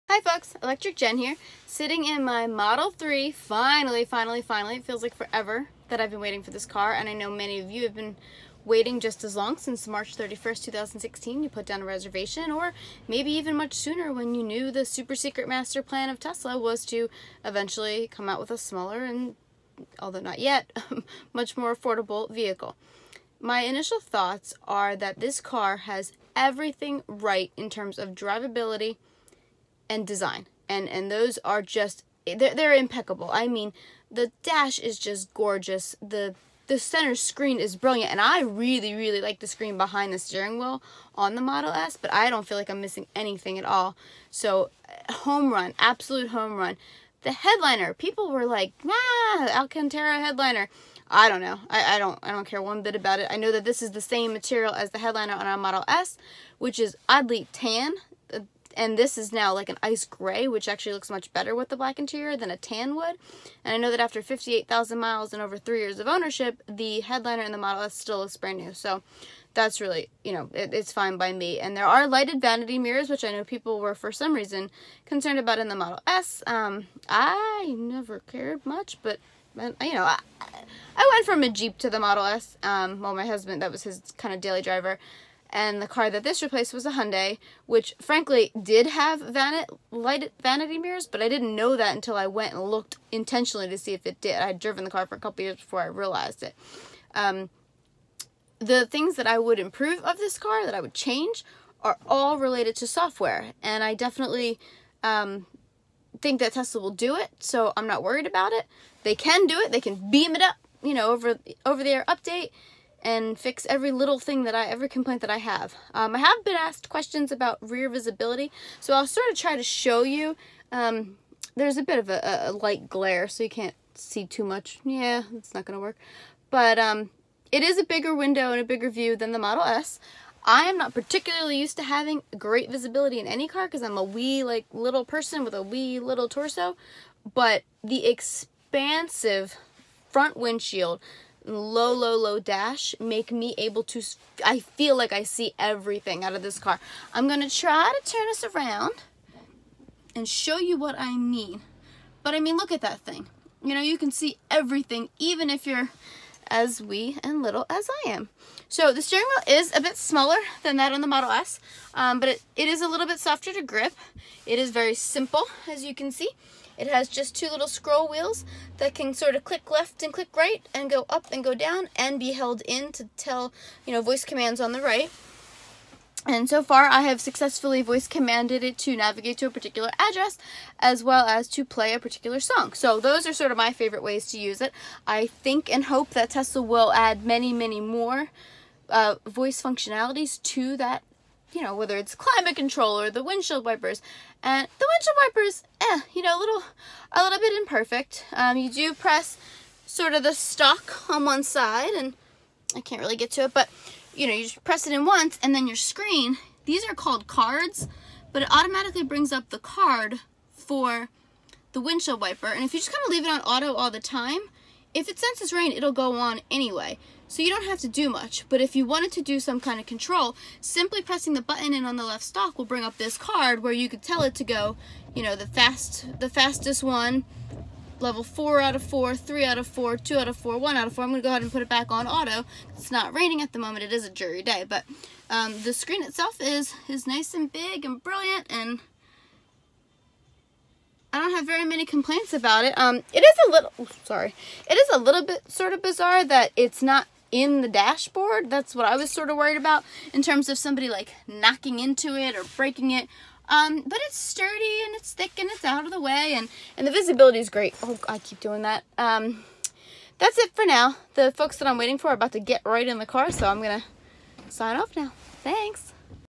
Model 3 First Impressions - 1 of 2
On the fly while I was waiting for a few folks, just blabbing a bit about my initial impressions.